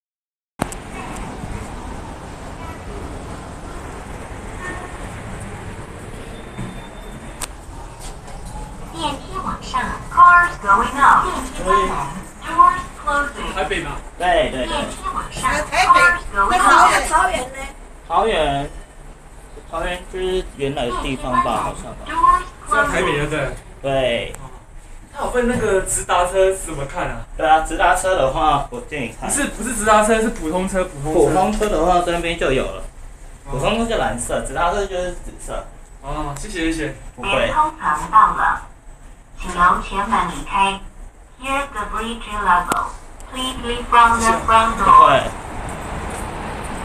桃園機場捷運A9林口站一月台電梯3F往上到4F天橋
拍攝時間:2017/3/3   18:13
我第一次拍桃園機場捷運的電梯